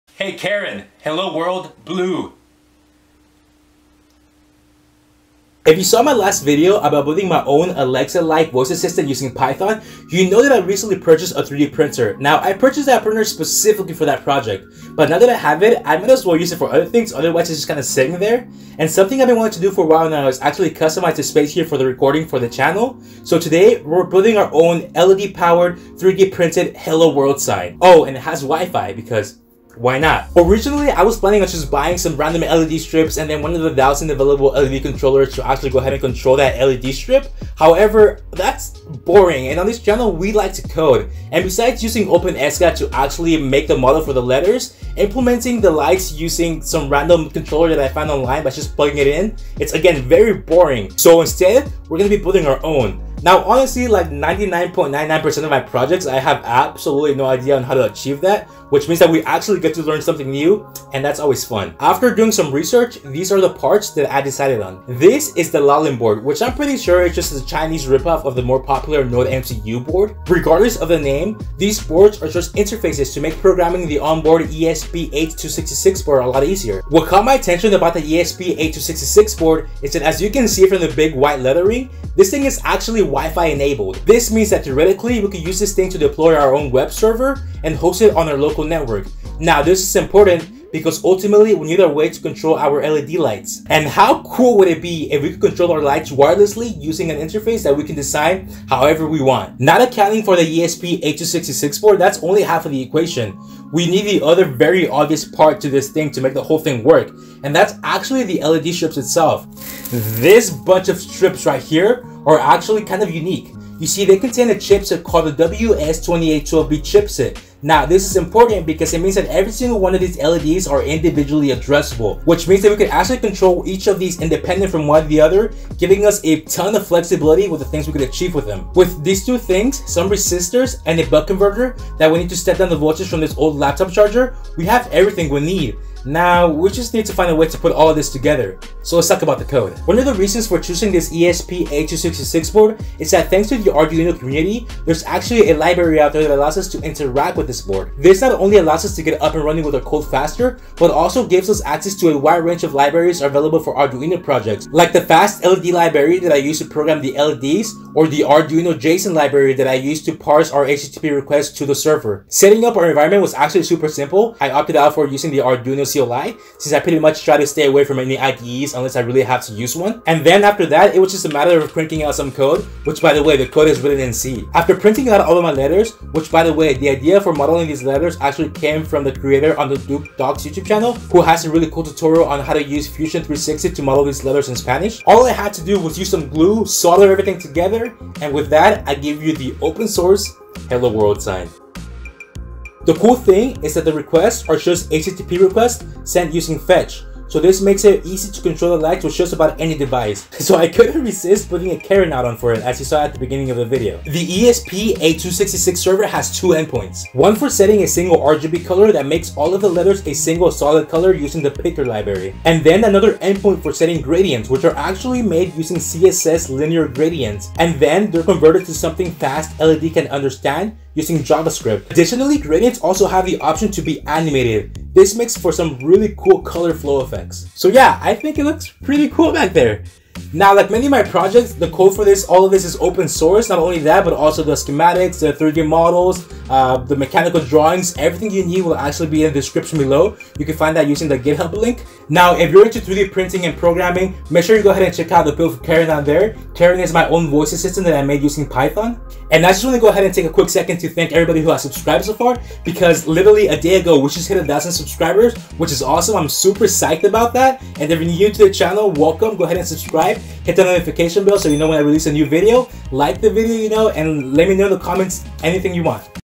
How to build an Arduino powered RGB LED Sign | Arduino 3D Printed RGB LED Hello World Sign with WIFI
I decided to build an ESP8266 Arduino powered RGB LED 3D printed sign. This build makes use of the L0L1n (NodeMCU) board to program some WS2812B Addressable RGB LED Strips. Because these LED lights are individually addressable, we are able to control them using the FastLED Arduino Library. We can also create a web server on the ESP8266 board to control the LEDs using a web app or any other device that is capable of sending HTTP POST requests to our ESP8266 server. Since the program is build using the Arduino CLI, all of the code that powers the server and controls the LEDs is written in C/C++. The web app that controls the LED lights is built using HTML, CSS, and vanilla
ECMAScript 6 (ES6) Javascript.

Gear:
📷 Canon EOS Rebel T7i
💻 Intel i7 XPS 13 16GB RAM (Arch Linux)